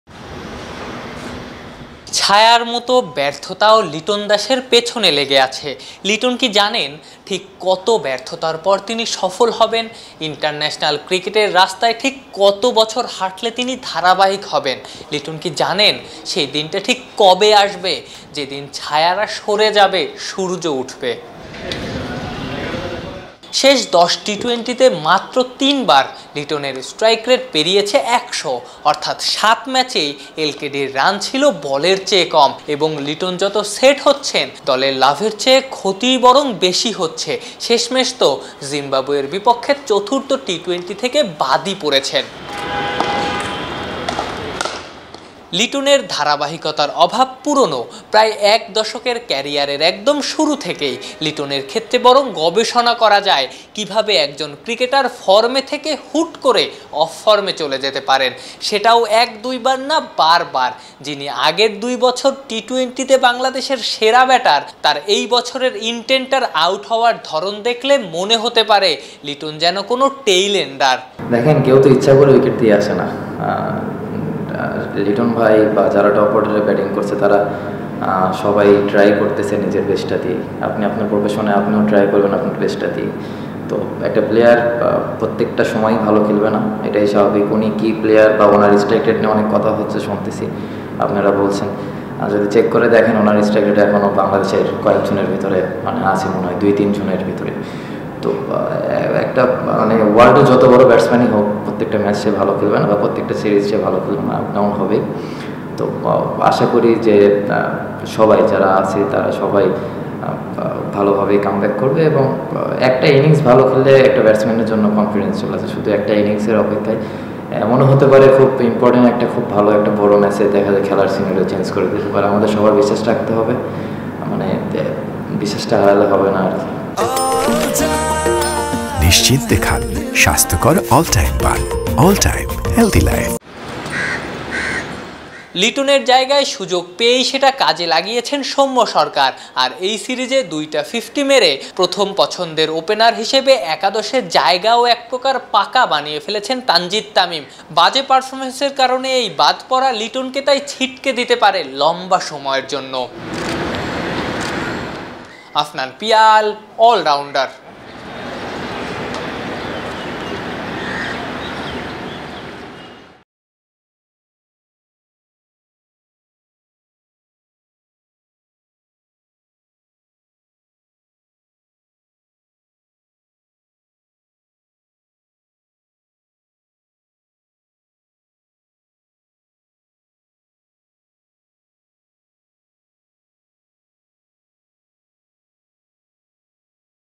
লিটনের পেছন থেকে ব্যর্থতার ছায়া কবে সরবে? তানজিদ-সৌম্য পারফর্ম করায় লিটন এখন থার্ড চয়েস ওপেনার
লিটনের পেছন থেকে ব্যর্থতার ছায়া কবে সরবে? তানজিদ-সৌম্য পারফর্ম করায় লিটন এখন থার্ড চয়েস ওপেনারGet in Touch: